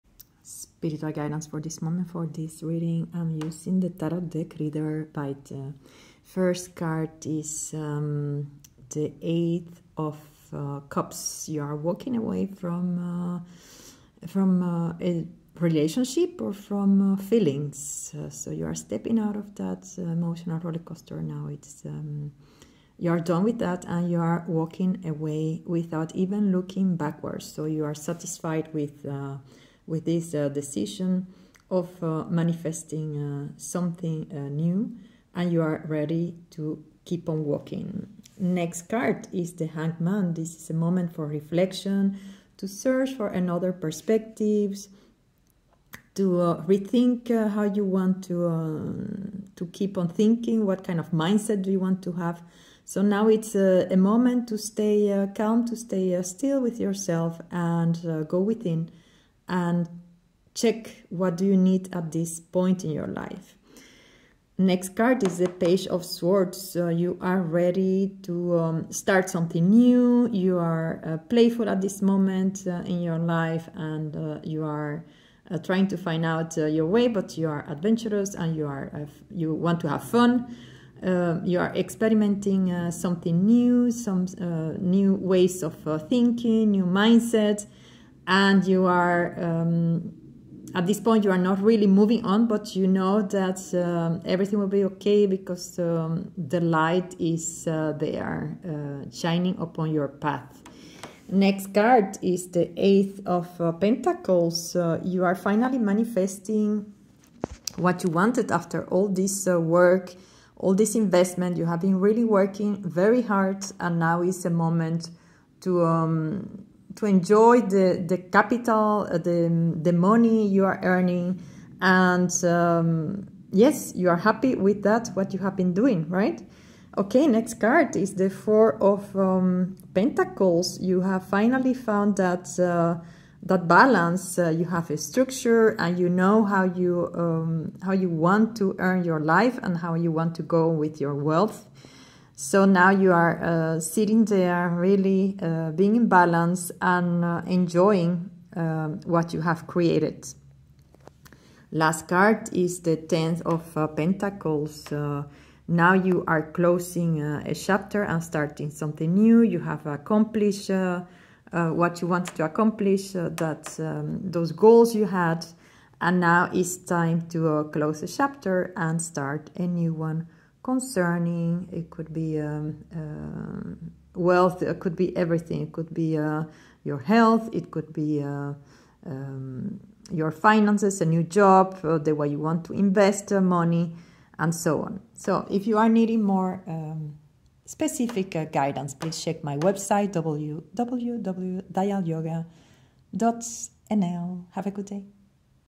Spiritual Guidance for Now ✨ Letting go, new perspectives on your way!
For this reading I'm using the Original Rider Waite Tarot Deck ✨

Take what resonates 🙌🌷

Namasté🙏
Doriana